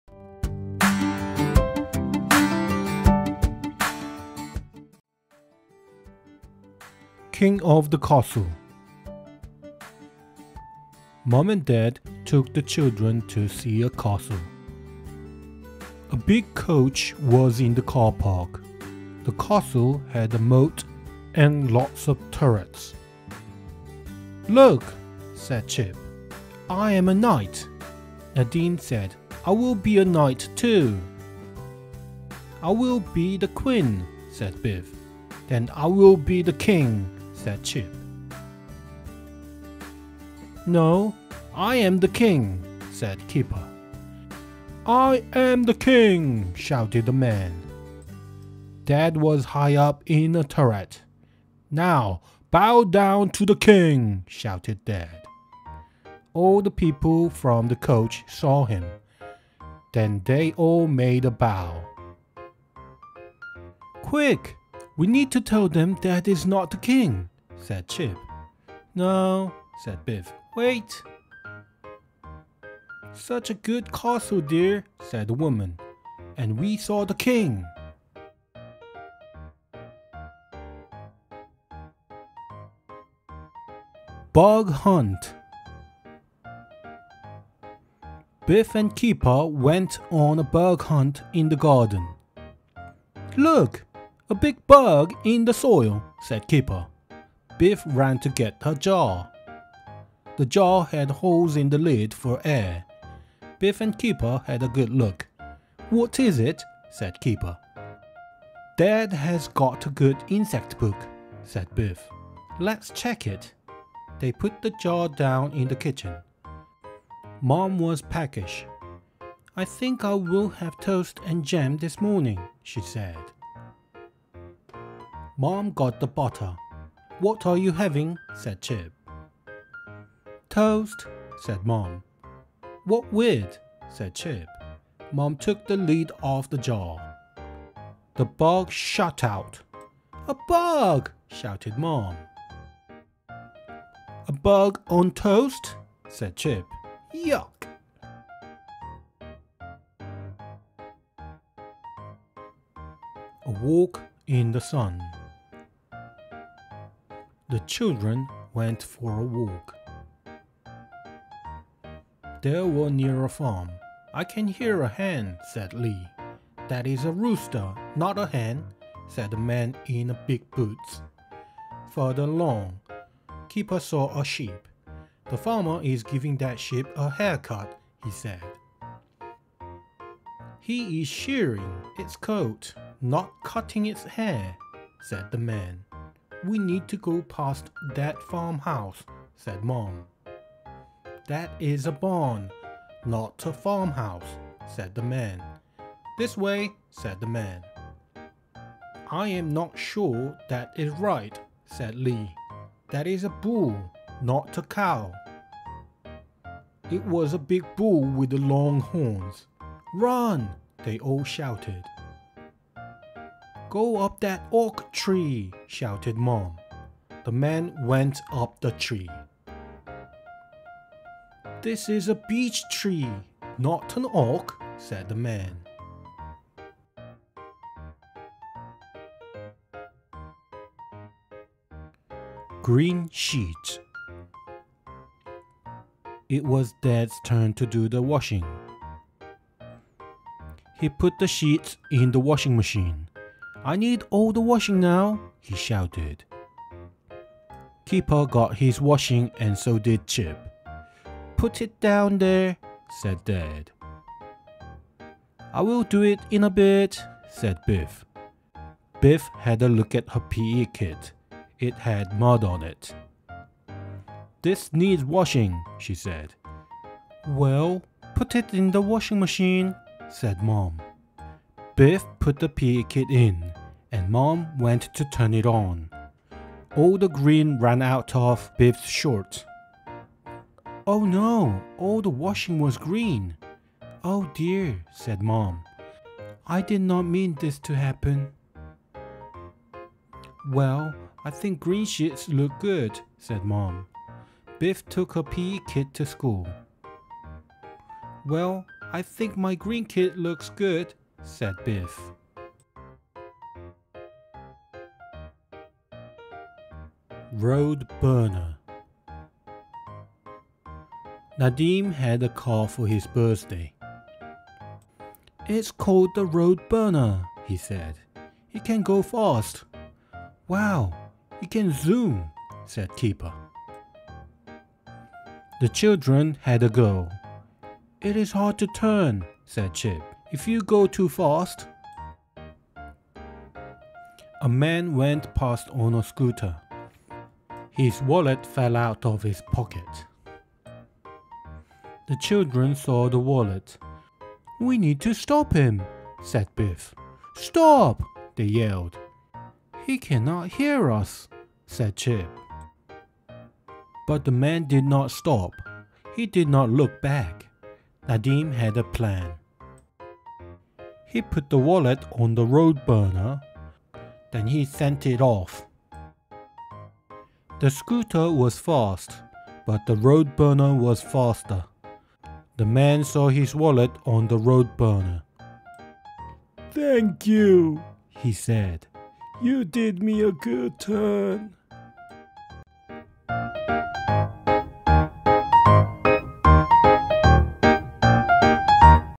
[ORT] {Stage 3} More A Decode and Develop 5권
ORT Stage 3 More A Decode and Develop 6-1 =5권
1. King of the Castle
2. Bug Hunt
3. A Walk in the Sun
4. Green Sheets
5. Road Burner

오디오 북/ Audio Book